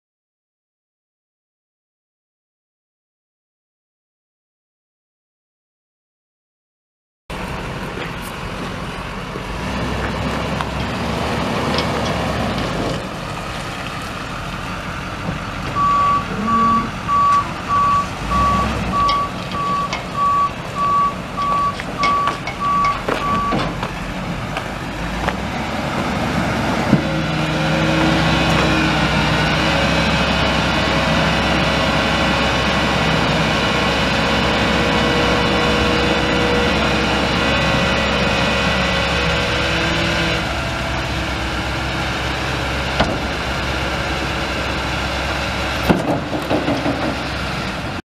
2006 Chevrolet C5500 dump truck for sale | no-reserve Internet auction March 20, 2018
Government Auction
View a detailed listing of this item and place bids

2006 Chevrolet C5500 dump truck
40,315 miles on odometer
6.6L V8 OHV 32V turbo diesel engine
Automatic transmission
Single axle
AC and heat
AM/FM
Suspension seat
PTO
Monroe steel dump bed
10'L x 8'W
24" side height
12" side board height
Pintle hitch
Spring suspension
Hydraulic brakes
225/70R19.5 tires
GVWR 19,500 lbs
Missouri title
1GBE5C1216F408171
It's located in Hannibal, MO, and we're selling it to the highest bidder regardless of price.